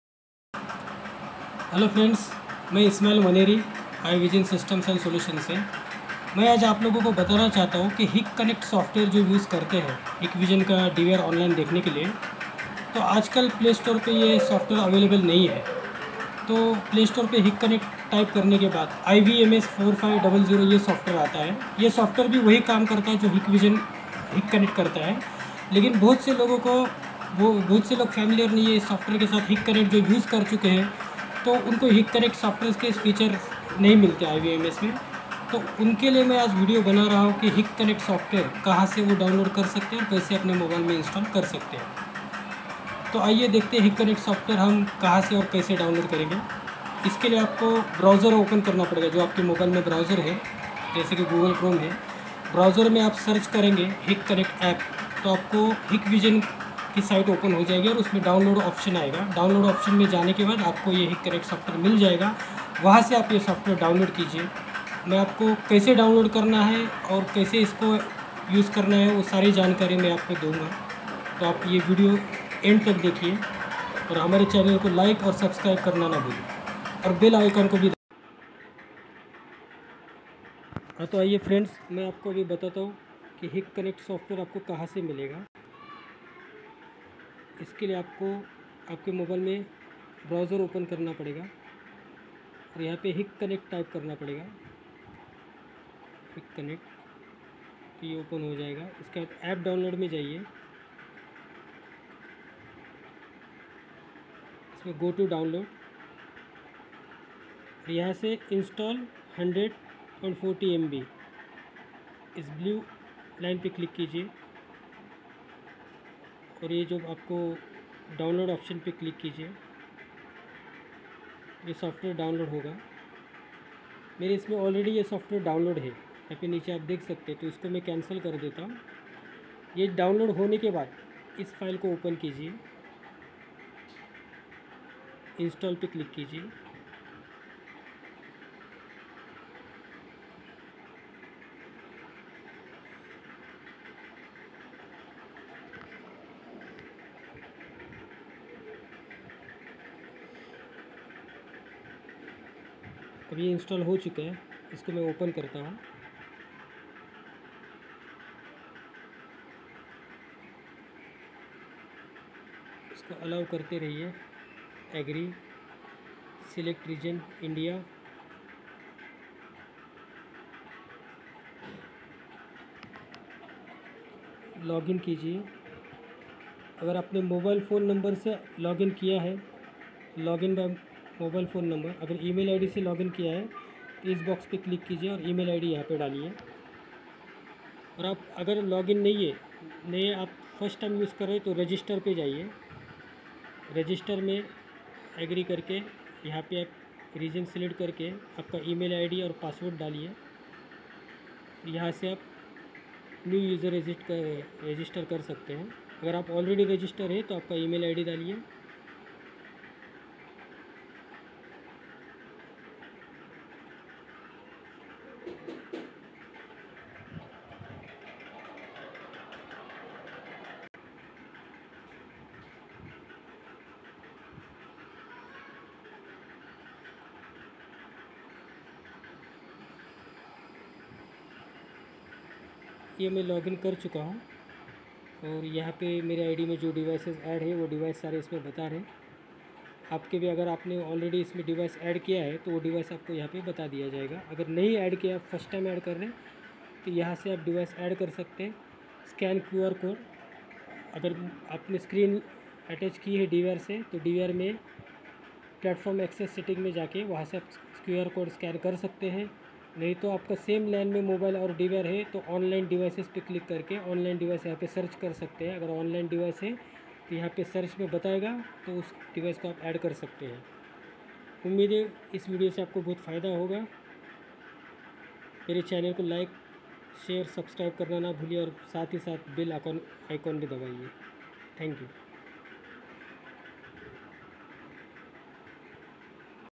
how to download hik connect app | I Vision systems and solutions
how to download hik connect app in android

hikconnect aap play store par available nahi hai to is video me maine bataya hai ki wo kaha se aur kaise apane mobile me download karana hai